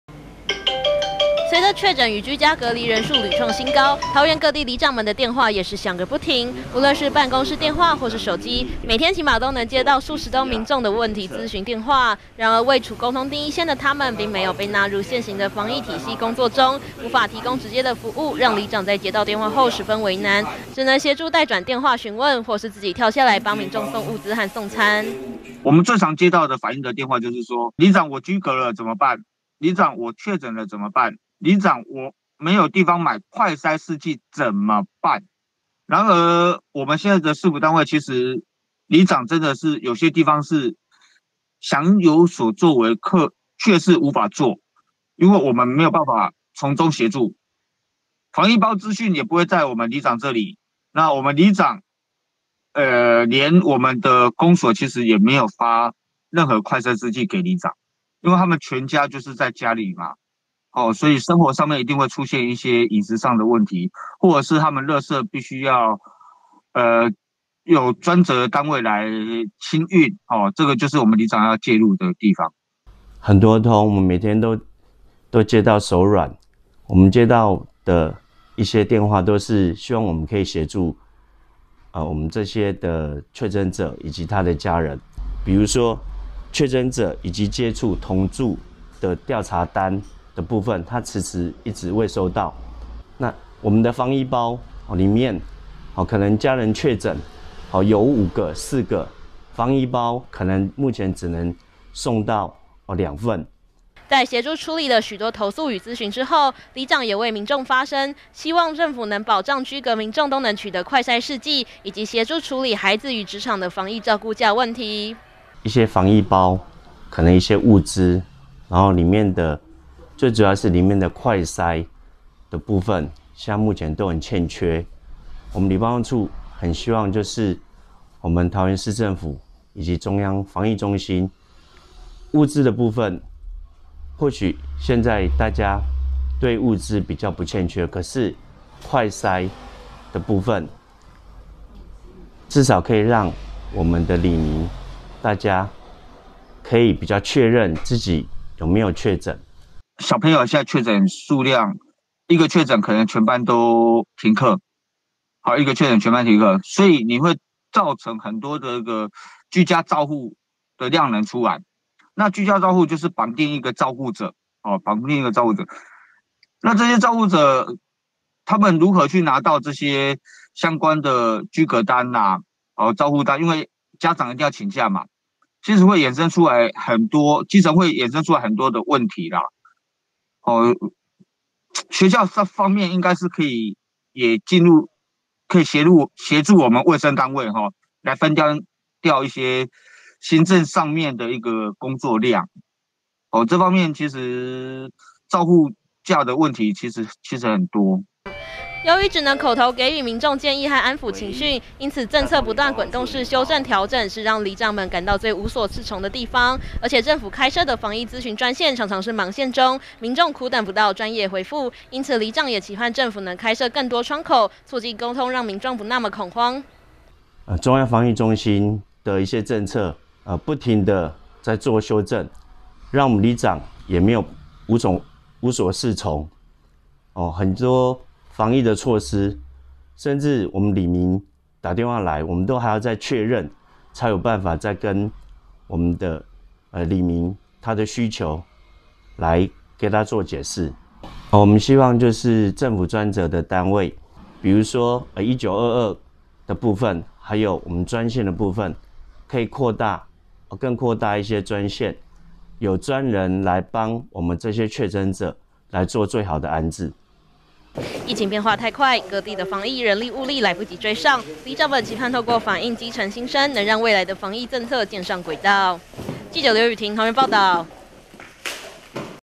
疫情 亂象|防疫諮詢要問誰? 里長為難盼多設窗口
進來疫情升溫里長們每天接數十通電話因此期盼反應基層所見疫情亂象盼政策早日步上正軌

隨著疫情延燒，政府開設的防疫諮詢專線常常是忙線中，也讓各地里長每天都有接不完的民眾諮詢電話，但由於里長並未被納入現行防疫體系的工作中，加上政策一直在滾動式修正，因此里長也感到十分為難，期盼政府能廣設諮詢窗口，提供民眾專業回覆。